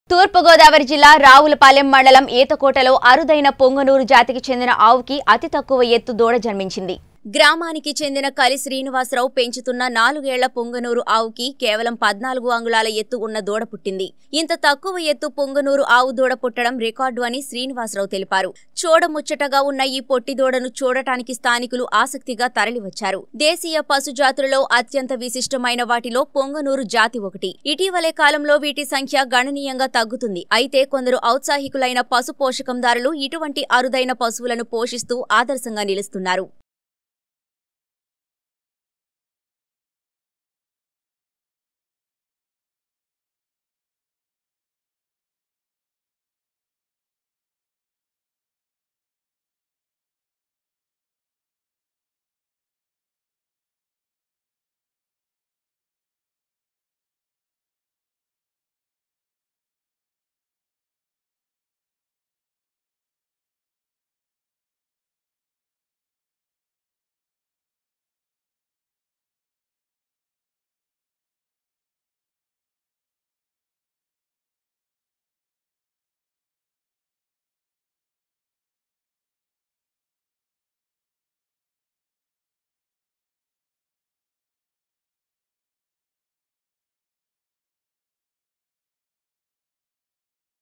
ఈతకోటలో అరుదైన పుంగనూరు జాతి చెందిన ఆవుకి జన్మించిన అతి తక్కువ ఎత్తుగల దూడ - రావులపాలెం
ఈతకోటలో అరుదైన పుంగనూరు జాతి చెందిన ఆవుకి జన్మించిన అతి తక్కువ ఎత్తుగల దూడ - రావులపాలెం | 19 Newstelugu